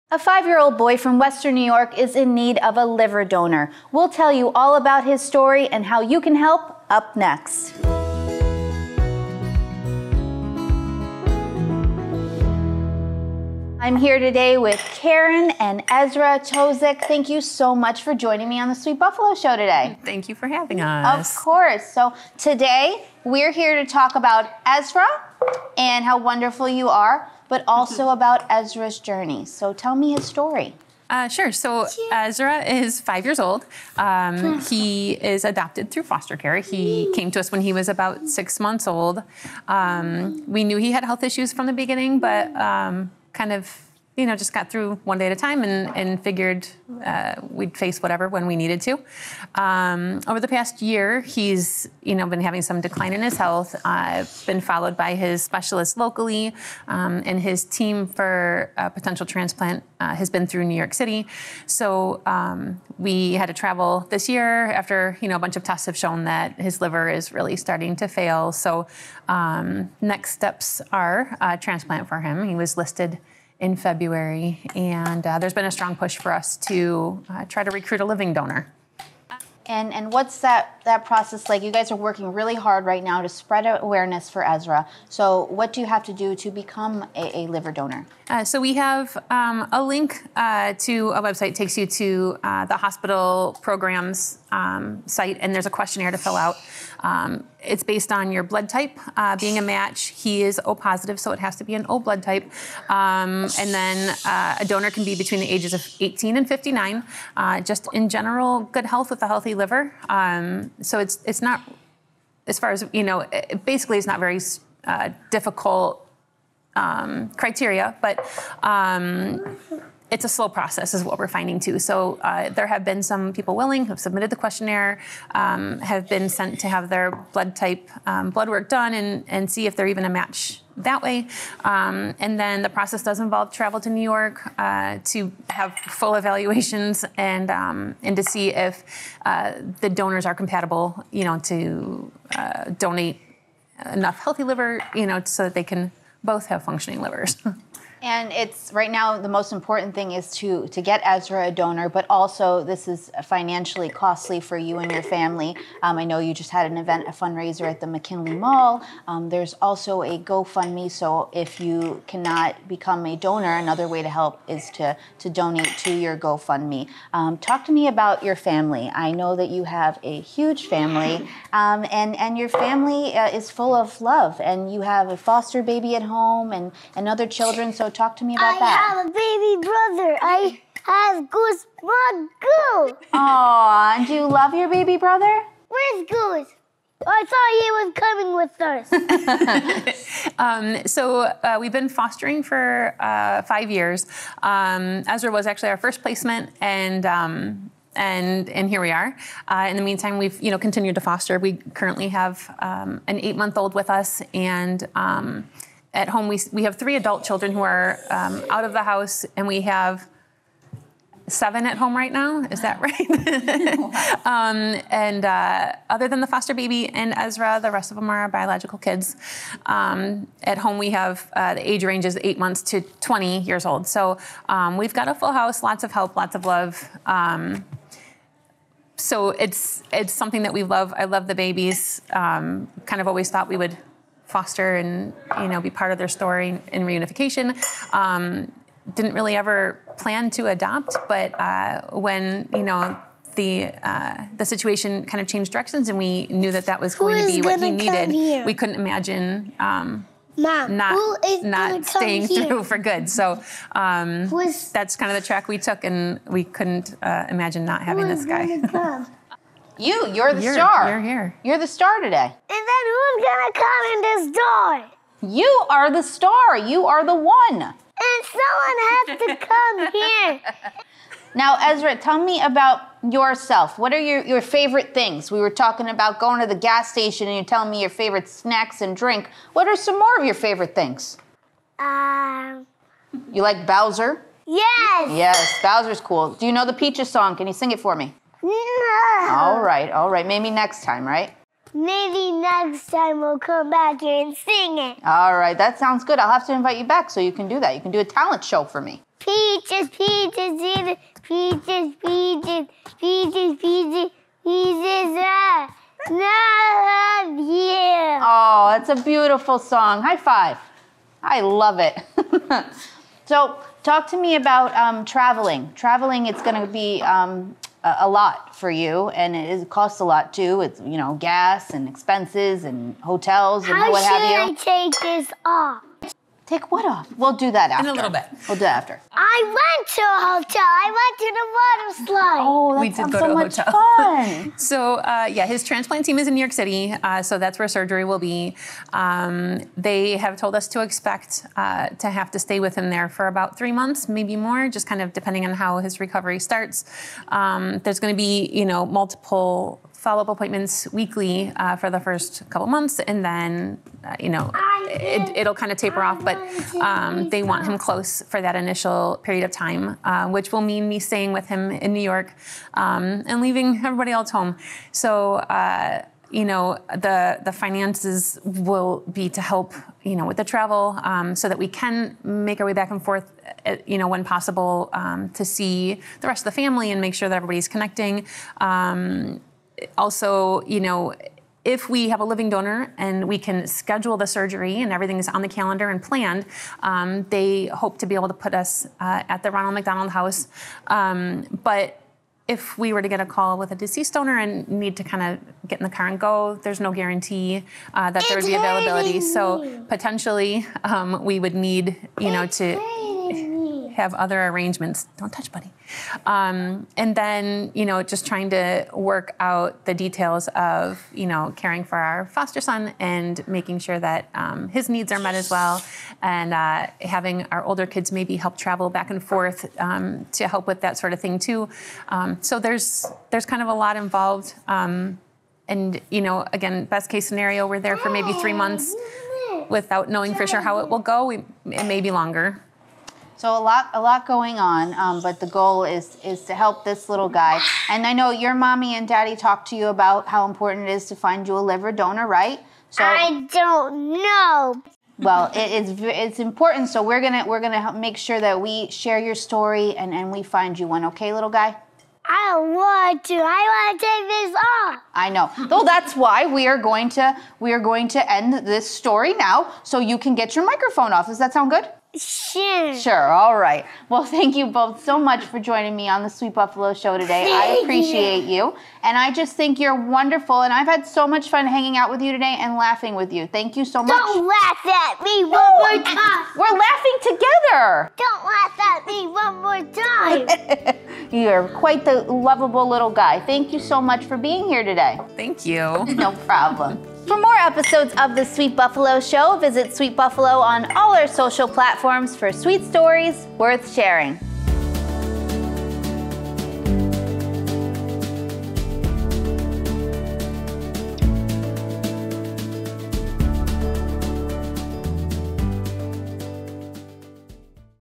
Five-year-old boy named Ezra in need of a liver donor
Sweet 5-year-old Ezra was in need of a liver donor. We're excited to share he found one!